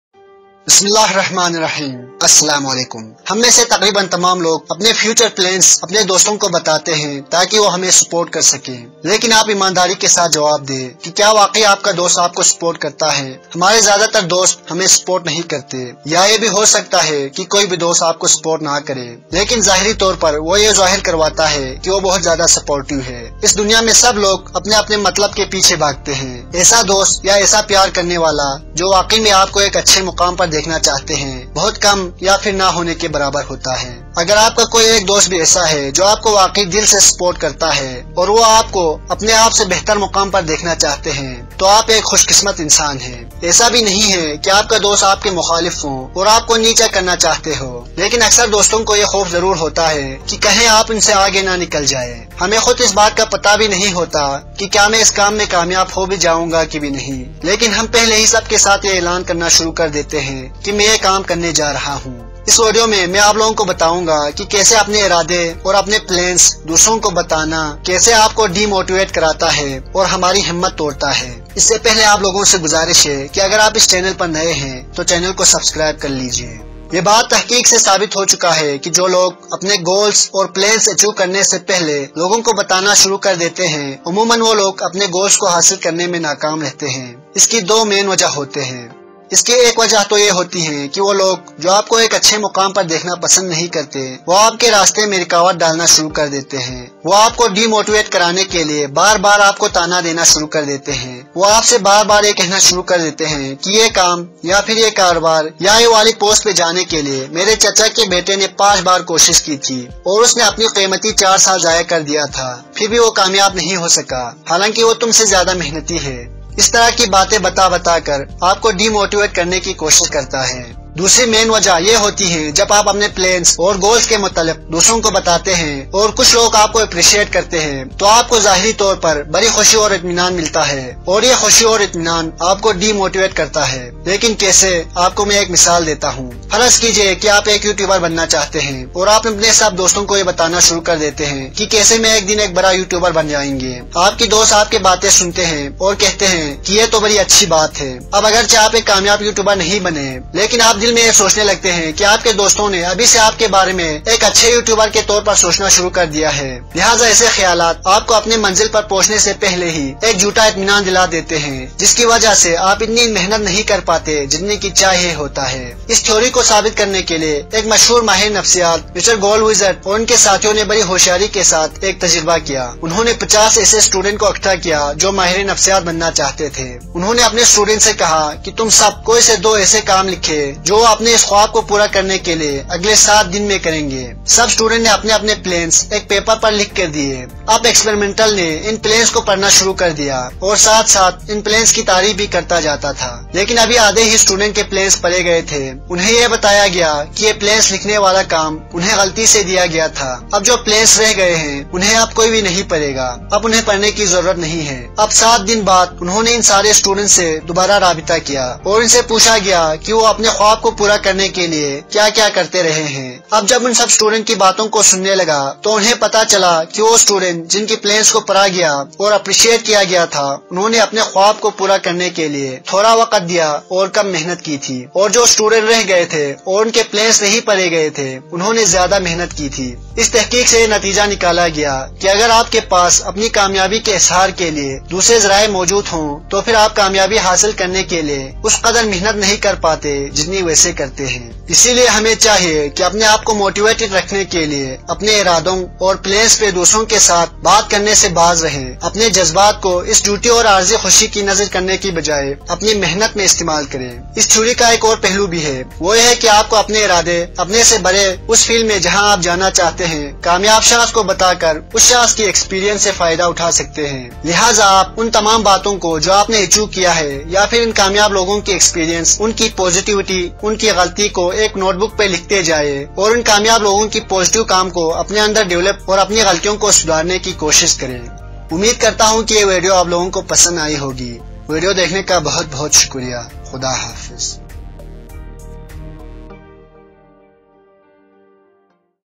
Bad effects of telling people about your future plains in urdu
Goal setting in urdu Hindi
In this video According to famous physiologist mr Gollwitzer some bad effects of telling your future plains to others